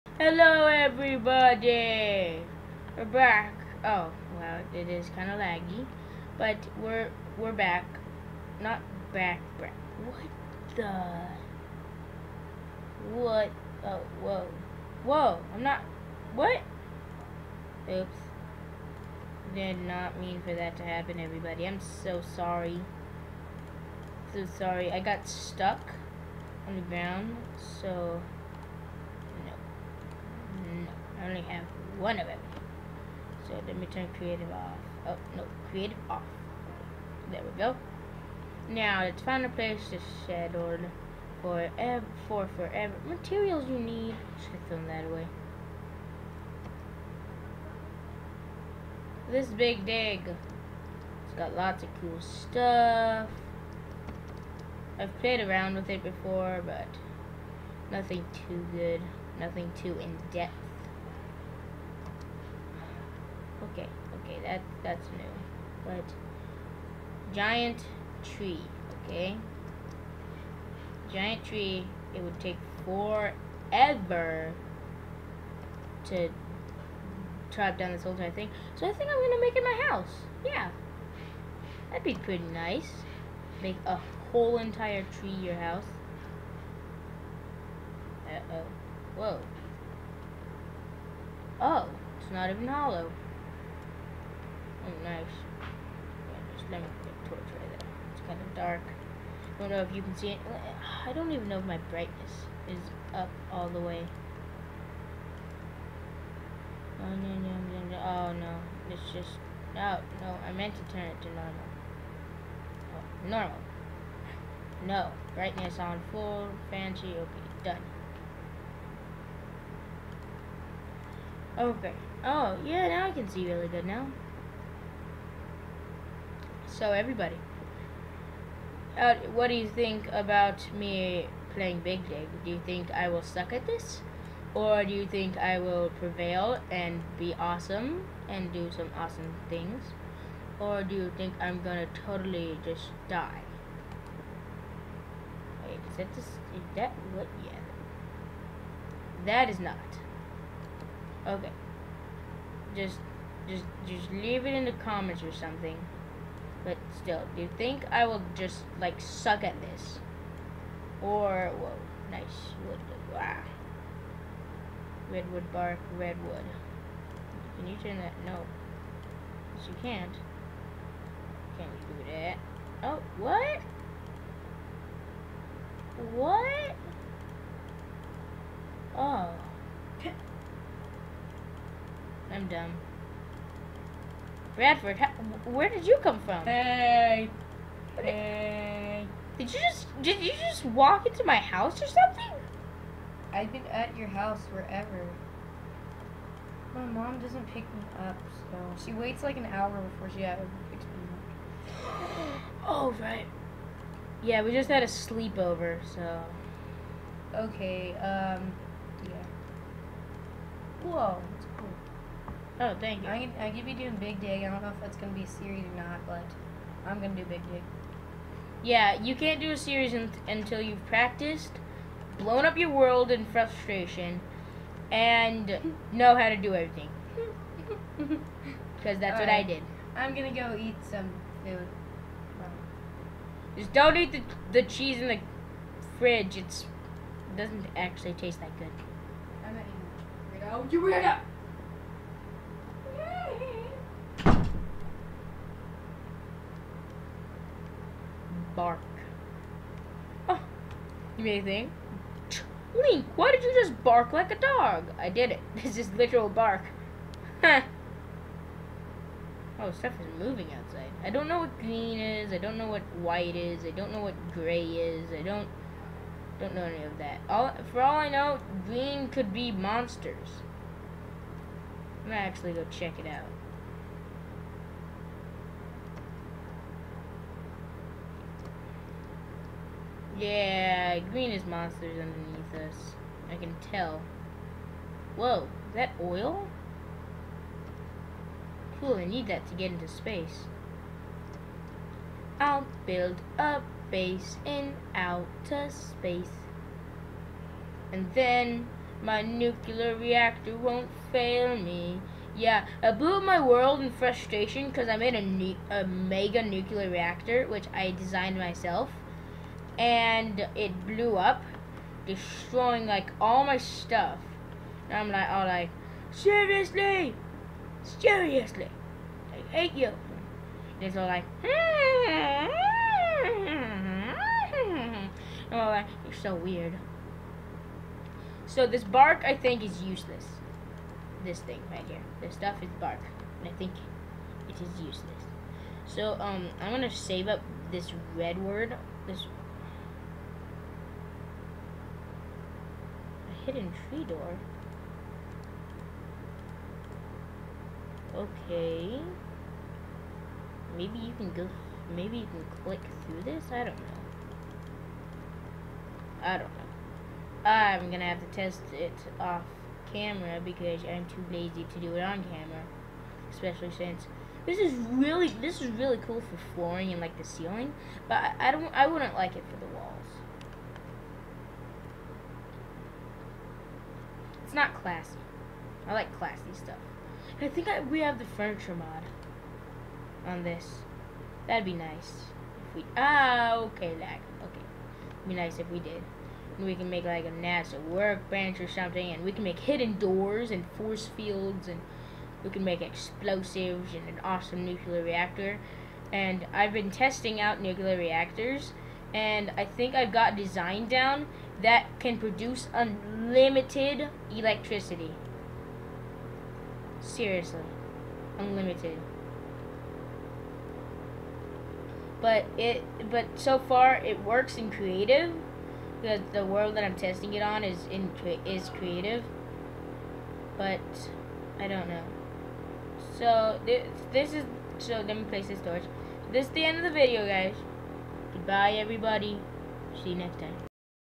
l&b solo link big dig #1 a giant tree!
ok i built a house in a tree!